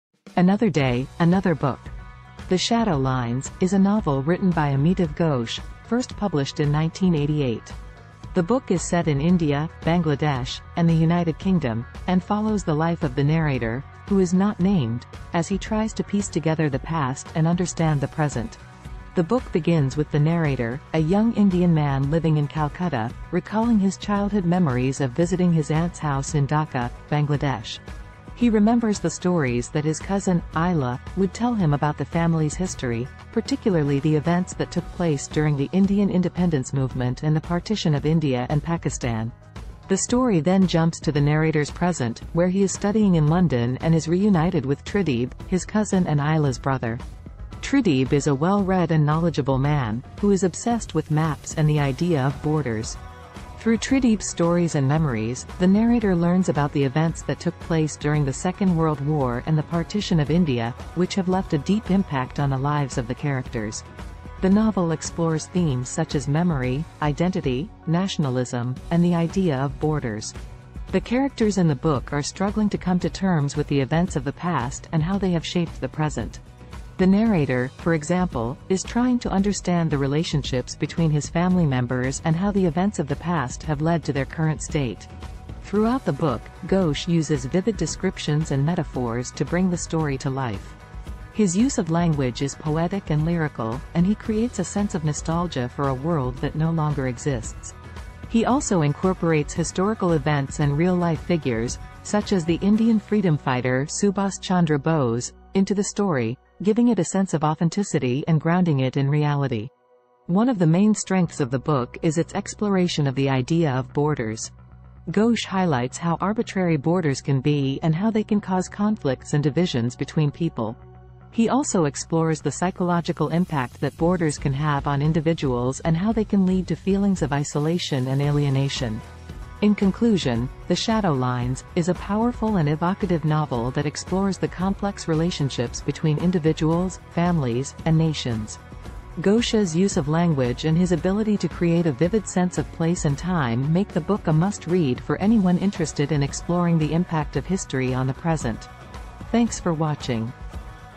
Another Day Another Book : "The Shadow Lines" by Amitav Ghosh
"The Shadow Lines" is a novel written by Amitav Ghosh, first published in 1988. The book is set in India, Bangladesh, and the United Kingdom, and follows the life of the narrator, who is not named, as he tries to piece together the past and understand the present.

The book begins with the narrator, a young Indian man living in Calcutta, recalling his childhood memories of visiting his aunt's house in Dhaka, Bangladesh. He remembers the stories that his cousin, Ila, would tell him about the family's history, particularly the events that took place during the Indian Independence Movement and the partition of India and Pakistan.

The story then jumps to the narrator's present, where he is studying in London and is reunited with Tridib, his cousin and Ila's brother. Tridib is a well-read and knowledgeable man, who is obsessed with maps and the idea of borders. Through Tridib's stories and memories, the narrator learns about the events that took place during the Second World War and the partition of India, which have left a deep impact on the lives of the characters.

The novel explores themes such as memory, identity, nationalism, and the idea of borders. The characters in the book are struggling to come to terms with the events of the past and how they have shaped the present. The narrator, for example, is trying to understand the relationships between his family members and how the events of the past have led to their current state.

Throughout the book, Ghosh uses vivid descriptions and metaphors to bring the story to life. His use of language is poetic and lyrical, and he creates a sense of nostalgia for a world that no longer exists. He also incorporates historical events and real-life figures, such as the Indian freedom fighter Subhas Chandra Bose, into the story, giving it a sense of authenticity and grounding it in reality.

One of the main strengths of the book is its exploration of the idea of borders. Ghosh highlights how arbitrary borders can be and how they can cause conflicts and divisions between people. He also explores the psychological impact that borders can have on individuals and how they can lead to feelings of isolation and alienation.

In conclusion, "The Shadow Lines" is a powerful and evocative novel that explores the complex relationships between individuals, families, and nations. Ghosh's use of language and his ability to create a vivid sense of place and time make the book a must-read for anyone interested in exploring the impact of history on the present.